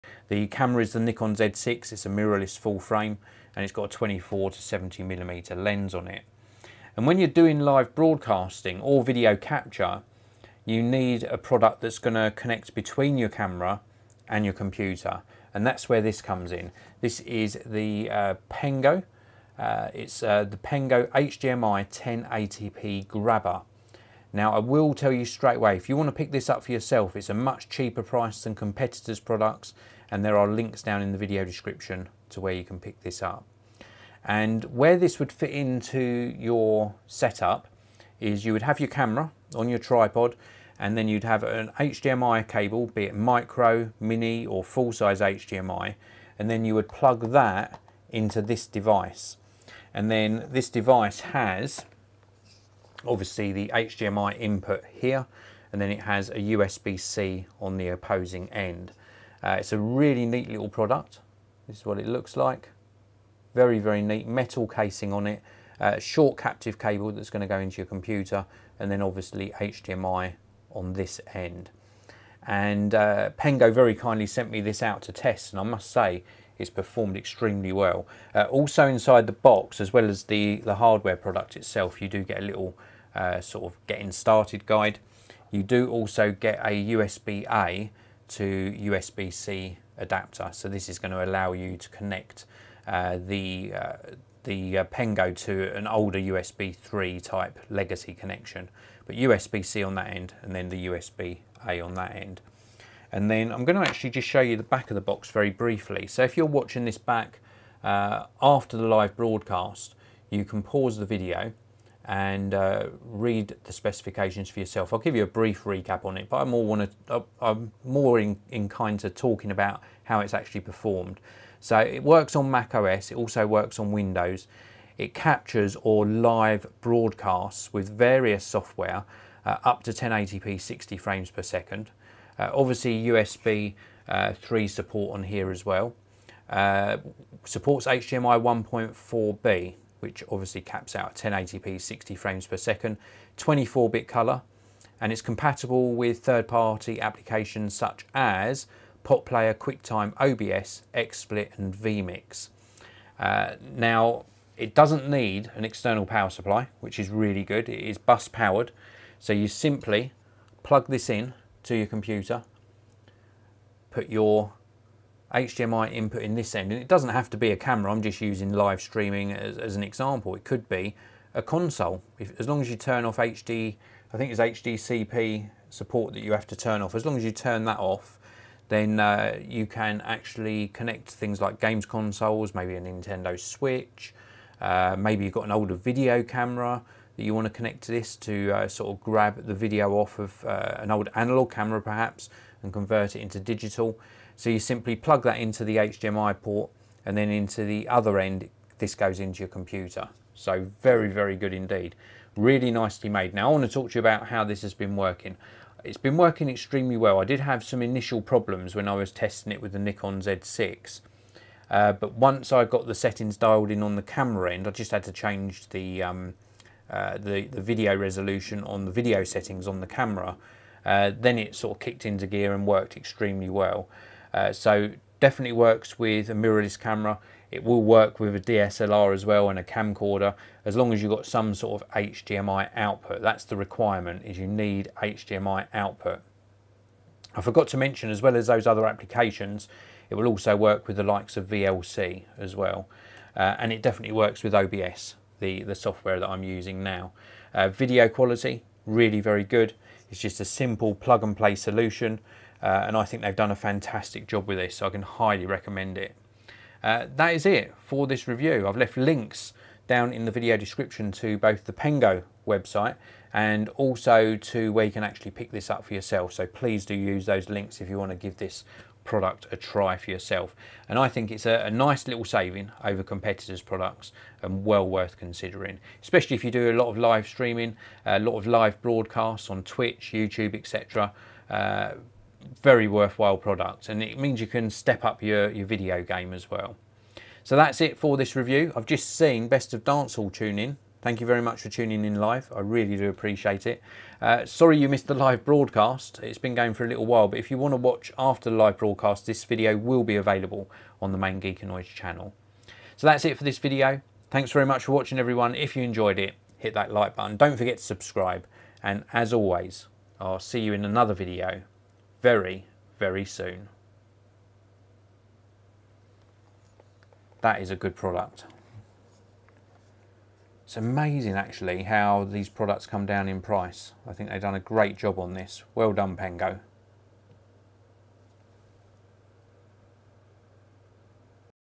Pengo HDMI 1080P Grabber Review
Easily captures videos up to 1080p@60fps via HDMI 1.4b standards such as Xbox, PS4 (turn off HDCP), Switch, Camcorders, DSLR and connects to your TypeC or USB 3.0 (adapter included) devices, such as laptops, desktops for final video editing. Like no other, no driver needed but will require a third party capturing app like OBS, VLC, Quicktime, vMix, XSplit, etc. Plug-n-play. No external power needed. Easily live stream via Youtube or Twitch.